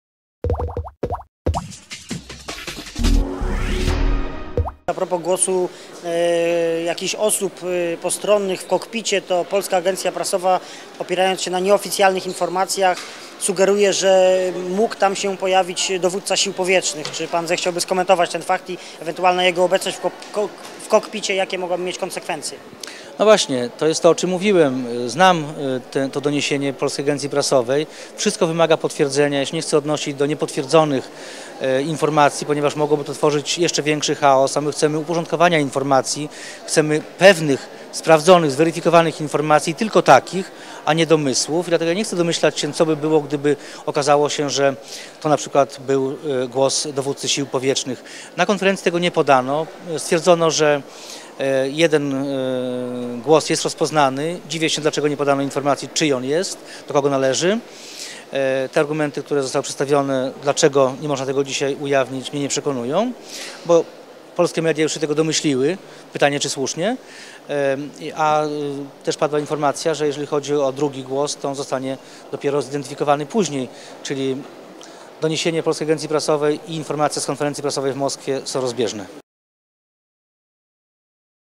Zieliński o "głosach w kokpicie": "Nie chcę się odnosić do niepotwierdzonych informacji"
plej.tv prezentuje: Jarosław Zieliński z PIS uważa, że aby móc skomentować doniesienia o tym, że tajemniczym głosem w kokpicie mógł być głosem dowódcy sił powietrznych, trzeba najpierw potwierdzić tą informację. Więcej wideo